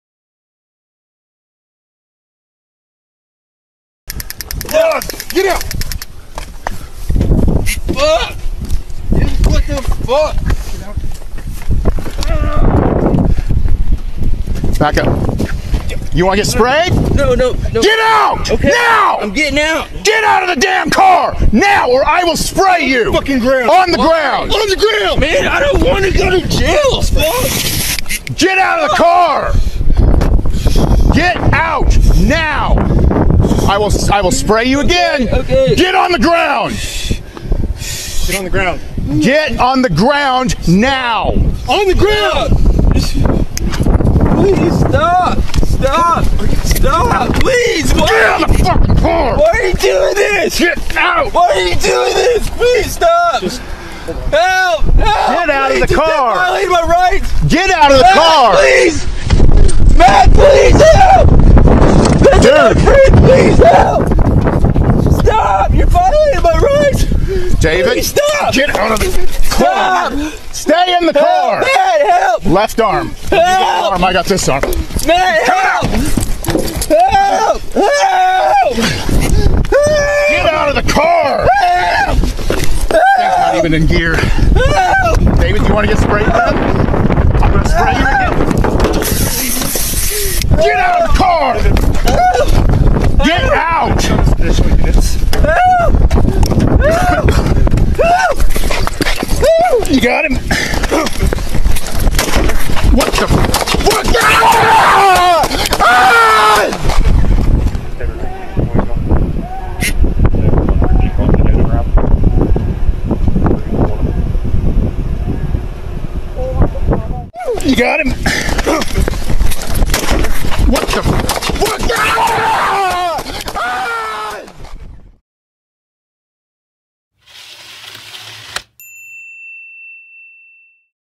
Guy Shoots Two Officers
Guy shoots 2 police officers

 What are your thoughts on this video?  is is resisting arrest or do you feel what he did was right? Their is no go or bad answer here.